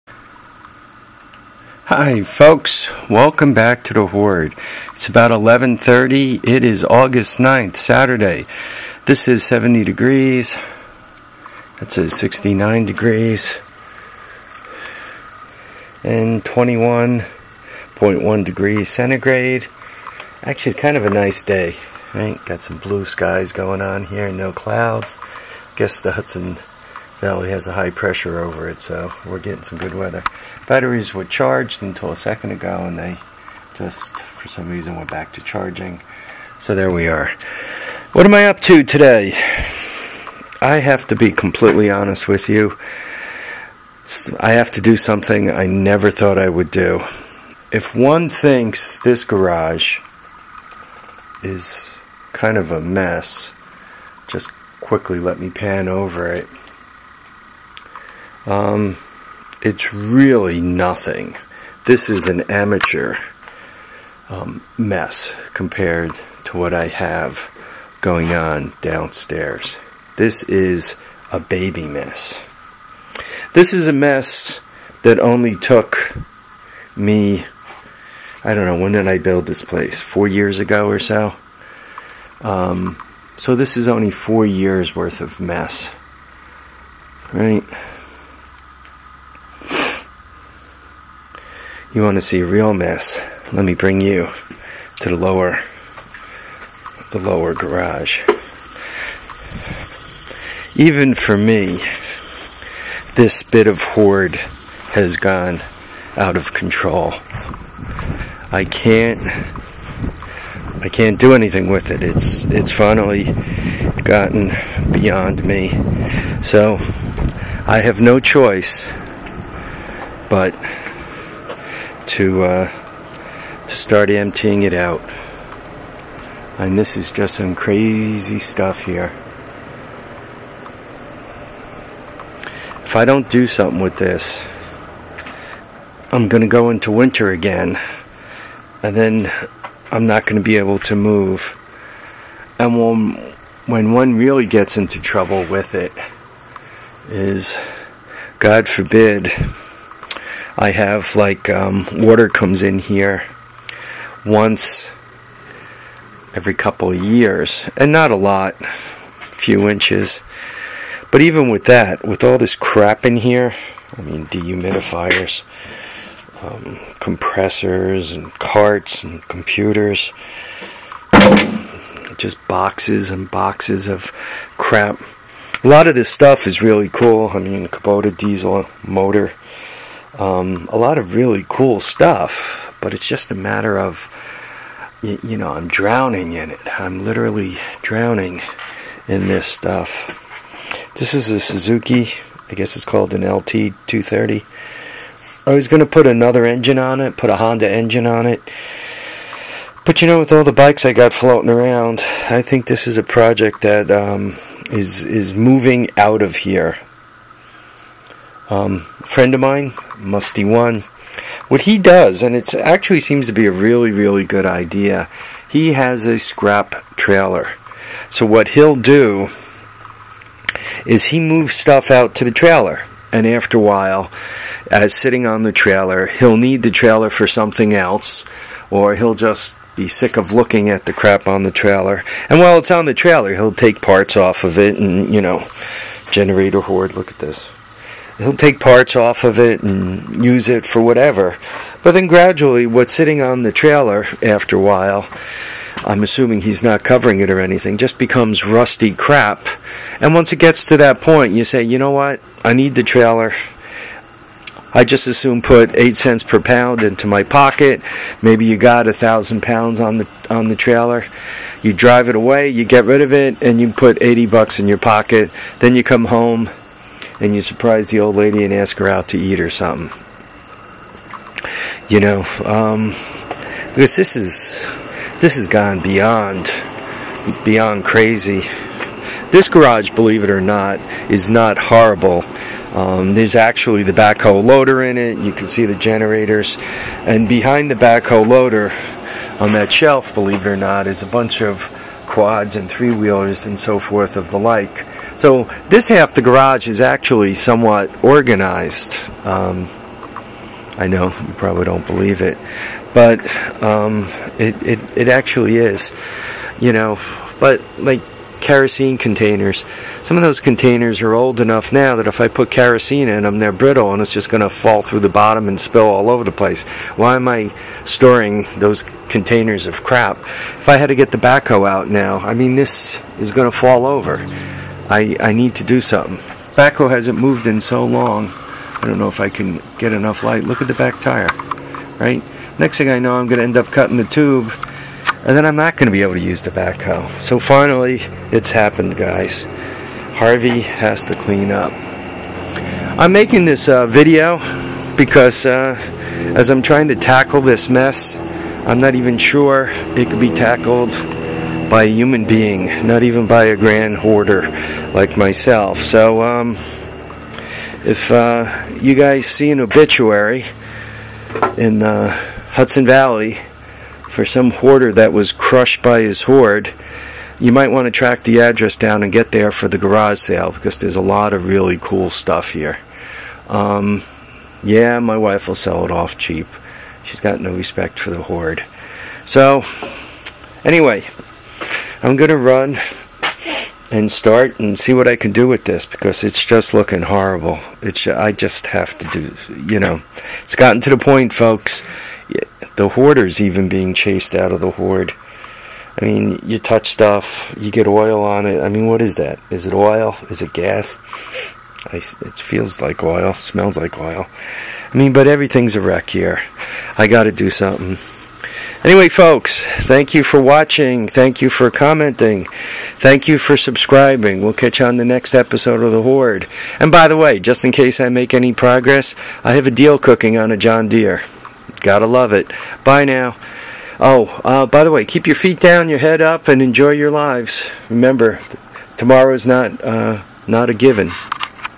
8_9_2014, Taming the lower Hoard? Part I
8_9_2014, Taming the lower Hoard?  Part I, Documenting the lower garage, before the clean up begins,.